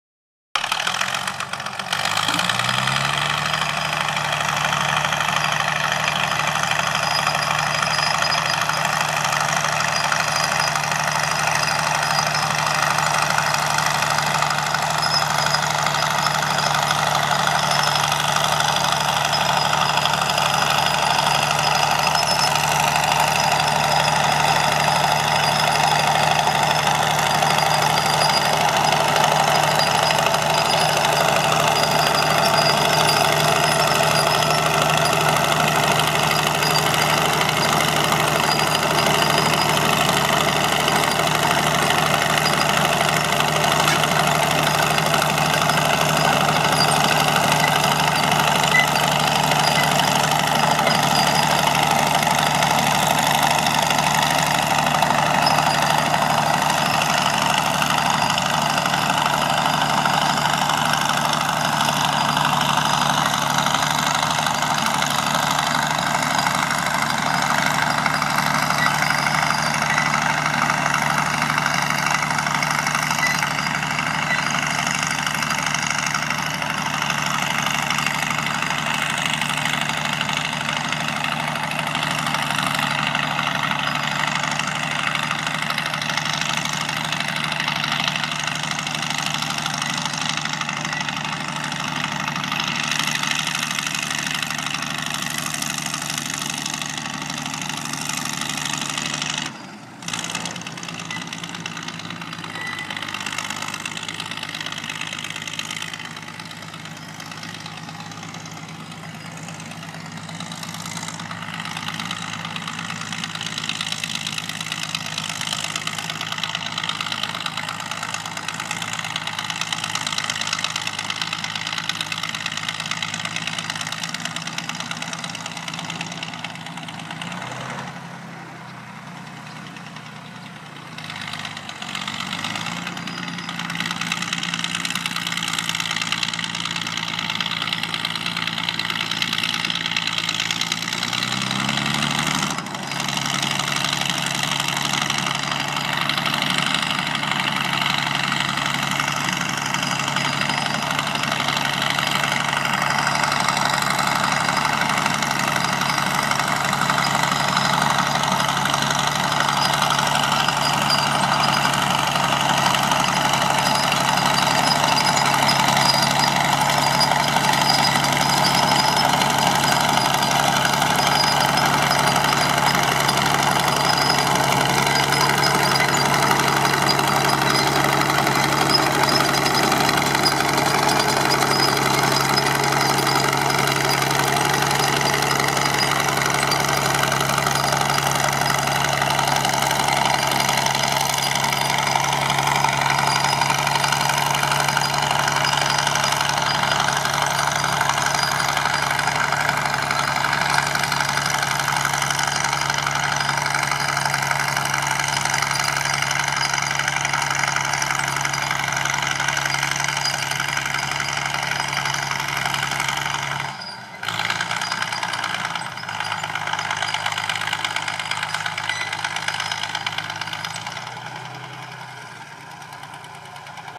FOWLER CHALLENGER 22.
Just restored. Trial run . This machine featured in December's issue of Tractor and Farming Heritage. 2013.